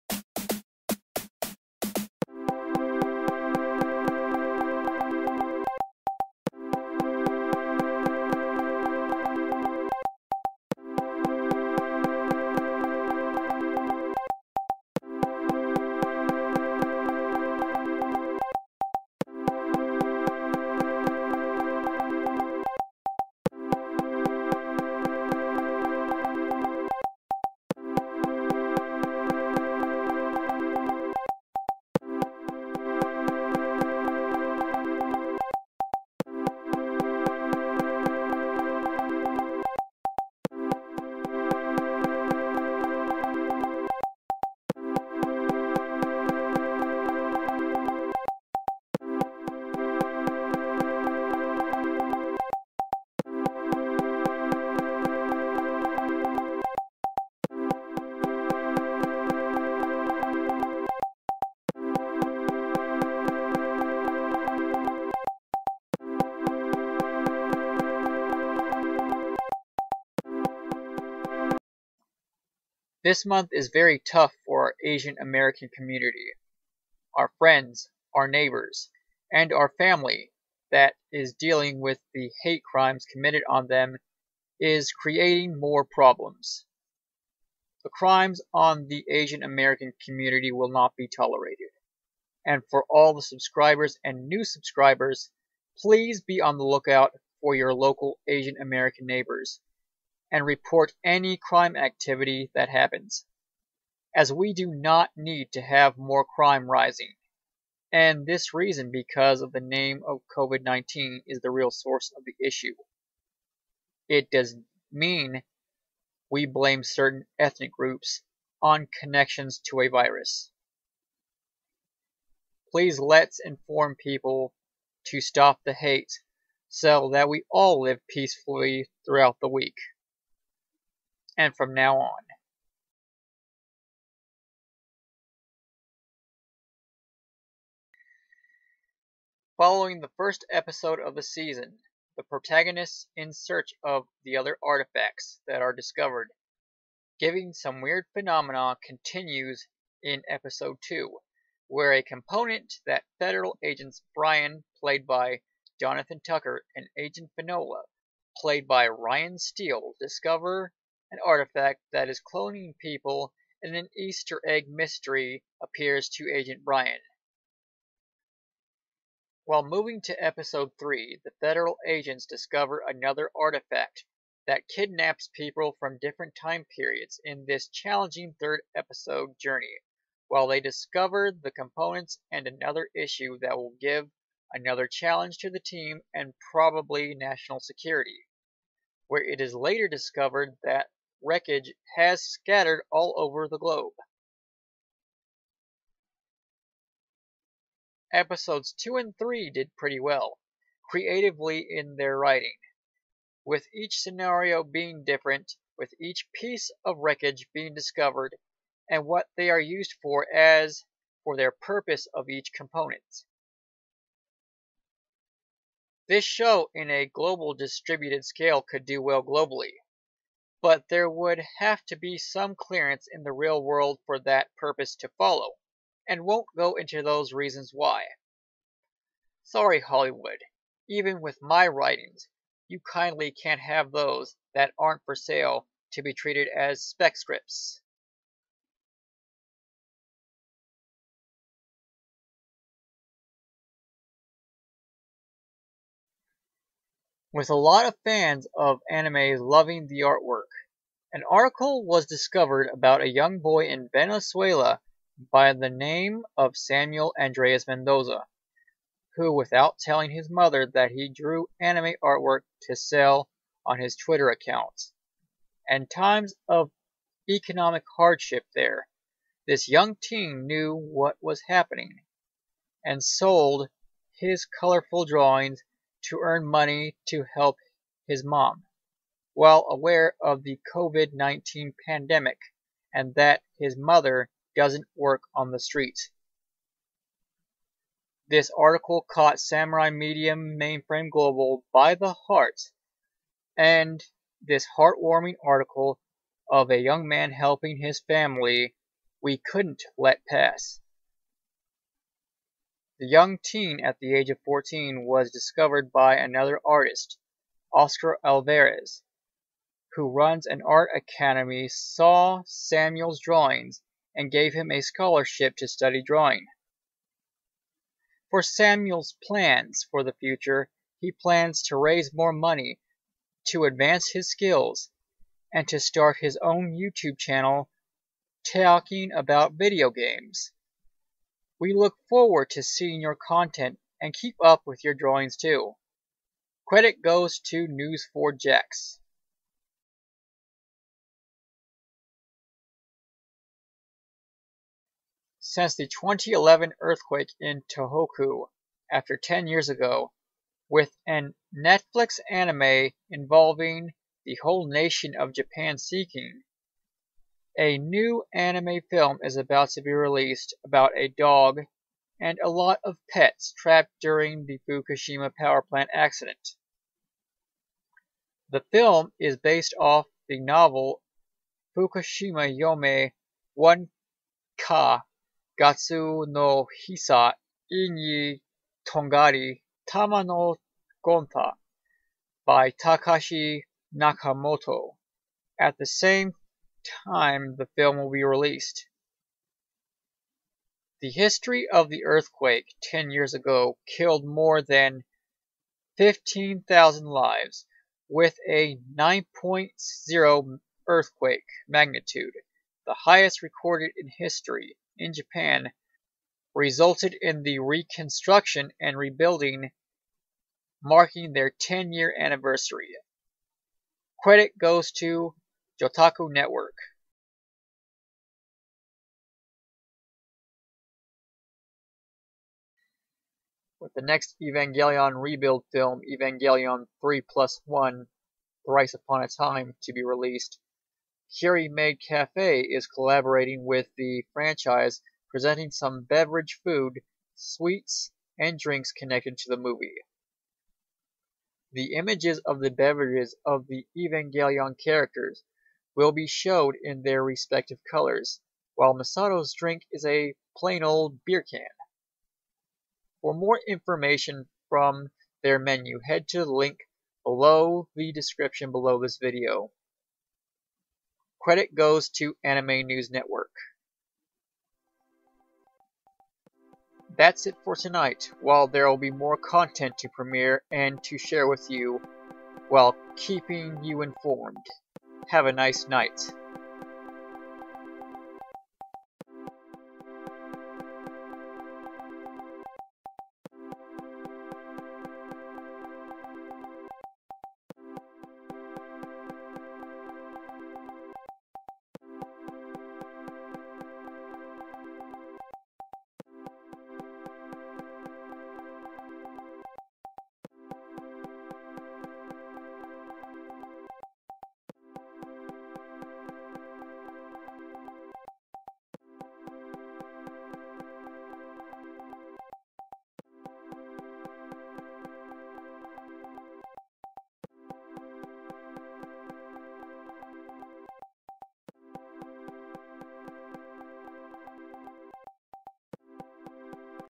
More on the Debris Journey and other Interesting News
THANK YOU FOR BEING GOOD LISTENERS OR FEEDBACK. STAY SAFE. STAY POSITIVE AND THINK POSITIVE!

More the show Debris while we walk about more anime.

Source

Venezuelan teen sells drawings...

New Anime Film Following a Dog and the Tohoku Earthquake...

Evangelion Collab Café Has the Most In-Character Drink...

Come Hang out at Discord!